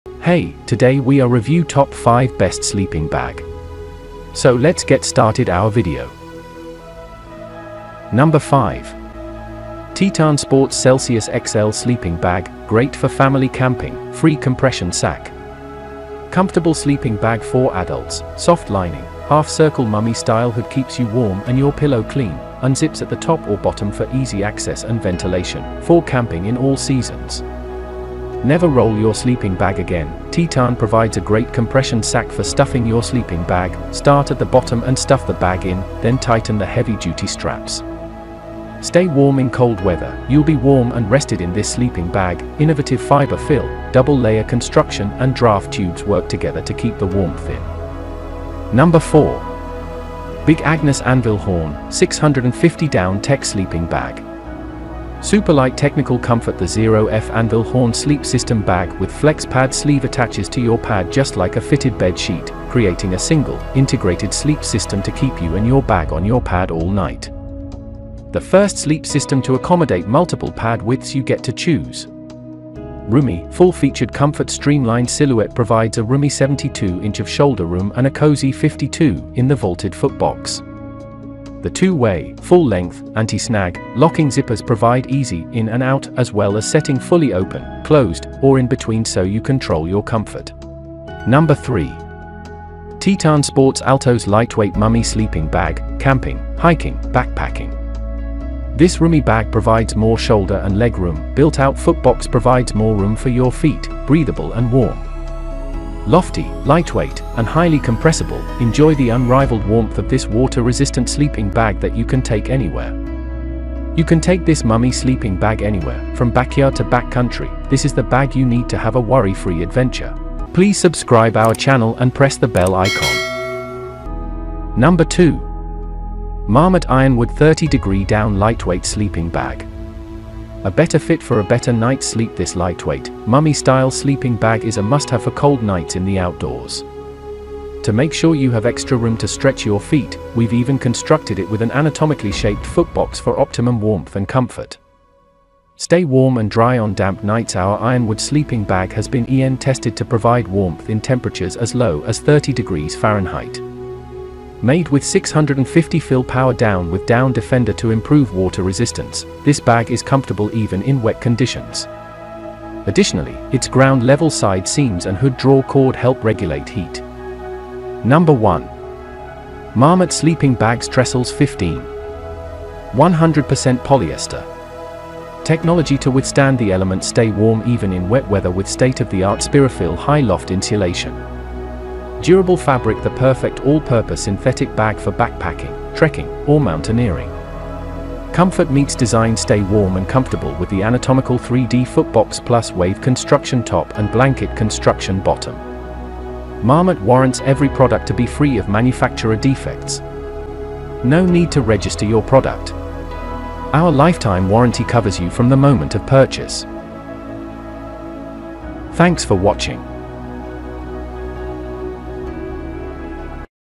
Top 5 Best Sleeping Bag Review In 2023
Check The Link for Buy & Details:
1. Adult Ultralight Mummy Goose Down Sleeping Bag

2. Naturehike Sleeping Bag Ultralight Waterproof Sleeping Bag

3. AEGISMAX Ultra-Light Adult Outdoor Camping Down Sleeping Bag

4. AEGISMAX G Series White Goose Down Mummy Camping Sleeping
Link: Very Warm White Goose Down Filled Adult Mummy Style Sleeping Bag

5. Very Warm White Goose Down Filled Adult Mummy Style Sleeping Bag